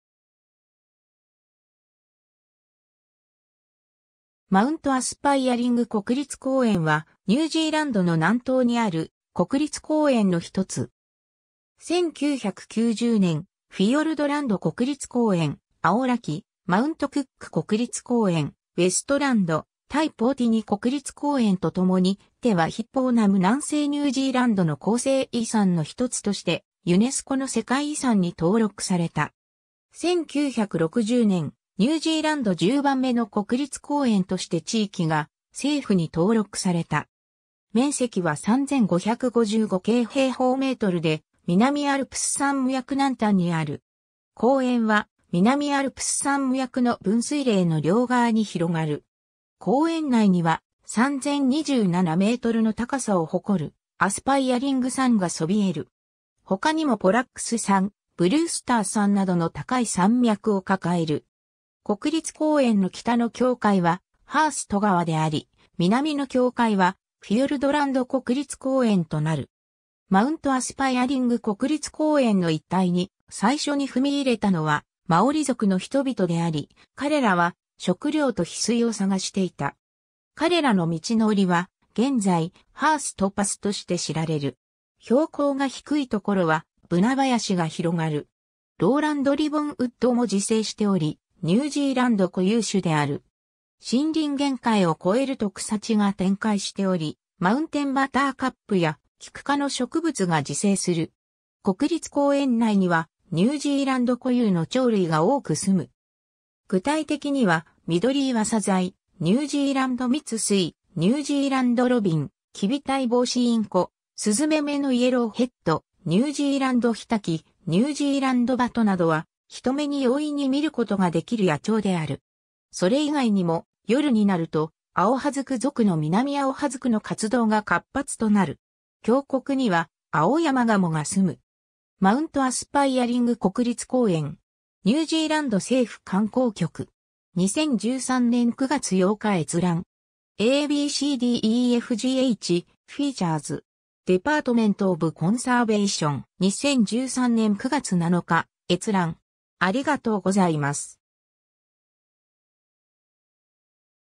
マウント・アスパイアリング国立公園
南島_(ニュージーランド)
マウント・アスパイアリング国立公園（Mount Aspiring National Park）は ニュージーランドの南島にある国立公園の一つ。
1990年、フィヨルドランド国立公園、アオラキ/マウント・クック国立公園、ウェストランド/タイ・ポウティニ国立公園とともに、テ・ワヒポウナム-南西ニュージーランドの構成遺産の1つとして、ユネスコの世界遺産（自然遺産）に登録された。
 1960年、ニュージーランド10番目の国立公園として地域が政府に登録された。
面積は3,555km²で南アルプス山脈南端にある。
公園は、南アルプス山脈の分水嶺の両側に広がる。
 公園内には、3027 mの高さを誇るアスパイアリング山がそびえる。
他にもポラックス山（2542m）、ブリュースター山（2519m）などの高い山脈を抱える。
 国立公園の北の境界は、ハースト川であり、南の境界は、フィヨルドランド国立公園となる。
 マウント・アスパイアリング国立公園の一帯に、最初に踏み入れたのは、マオリ族の人々であり、彼らは、食料とヒスイ（マオリ語:pounamu）を探していた。
彼らの道のりは、現在、ハースト・パスとして知られる。
 標高が低いところは、ブナ林が広がる。
ローランドリボンウッドも自生しており、ニュージーランド固有種である。
森林限界を越えると草地が展開しており、マウンテンバターカップやキク科の植物が自生する。
 国立公園内には、ニュージーランド固有の鳥類が多く住む。
具体的には、ミドリイワサザイ、ニュージーランドミツスイ、ニュージーランドロビン、キビタイボウシインコ、スズメ目のイエローヘッド（マオリ語：mohua）、ニュージーランドヒタキ、ニュージーランドバトなどは、人目に容易に見ることができる野鳥である。
それ以外にも、夜になると、アオハズク属のミナミアオハズクの活動が活発となる。
峡谷には、アオヤマガモが住む。
 ^ “マウント・アスパイアリング国立公園”. ニュージーランド政府観光局. 2013年9月8日閲覧。
 ^ a b c d e f g h “Features”. Department of Conservation. 2013年9月7日閲覧。